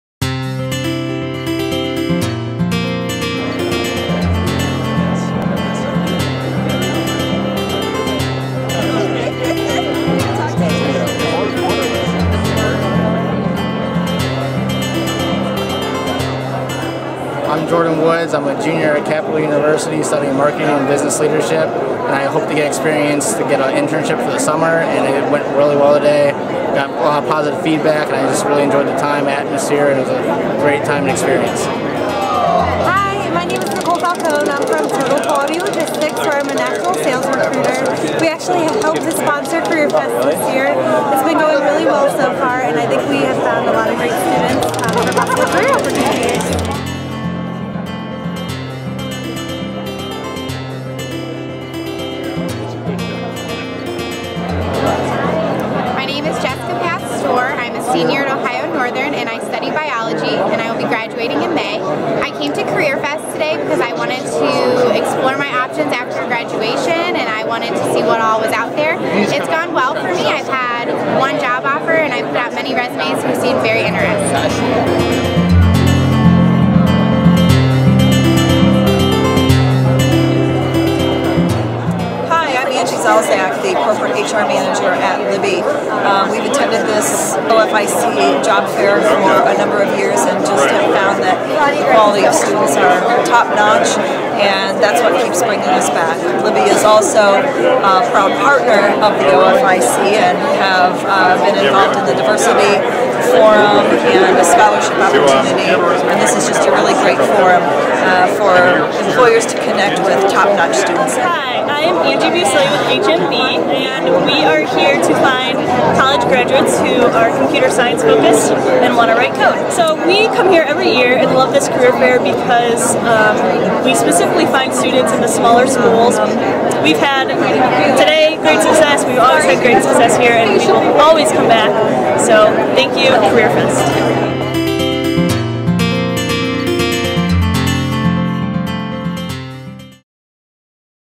CareerFest 2014 Highlights
CareerFest provides an efficient and enjoyable way for companies and organizations to address human capital needs. This is a great opportunity to meet students and recent graduates from 34 of Ohio's best colleges and universities, all of which are members of The Ohio Foundation of Independent Colleges (OFIC). If your organization is seeking quality interns, recent college graduates and alumni, this is THE career fair to attend!